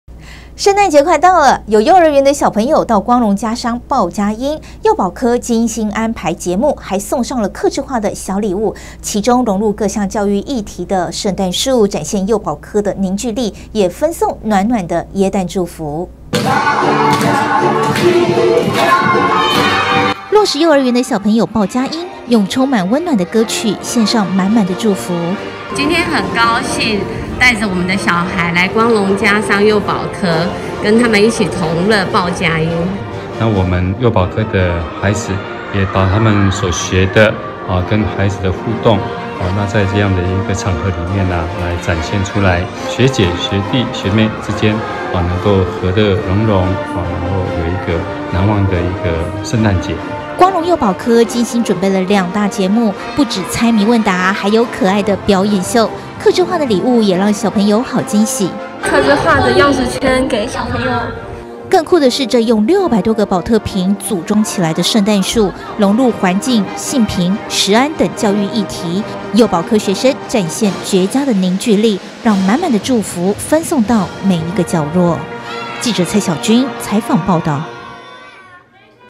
聖誕 報佳音|光隆、若石攜手歡度聖誕 環保聖誕樹永續傳情
(中嘉吉隆新聞 記者蔡曉君/基隆報導) 聖誕節快到了，若石幼兒園小朋友到光隆家商報佳音，光隆幼保科精心安排節目，還送上客製化小禮物，其中融入各項教育議題的聖誕樹，展現幼保科的凝聚力，也分送暖暖的耶誕祝福。

光隆幼保科精心準備了兩大節目，不只猜謎問答，還有可愛表演秀，客製化禮物也讓小朋友好驚喜。

更酷的是這用600多個寶特瓶組裝起來的聖誕樹，融入環境、性平、食安等教育議題，幼保科學生只限絕佳的凝聚力，讓滿滿的祝福分送到每一個角落。

臉書追起來
Home 中嘉- 吉隆有線電視

趣聞不漏接
吉隆官網(中嘉新聞)

喜歡看電視?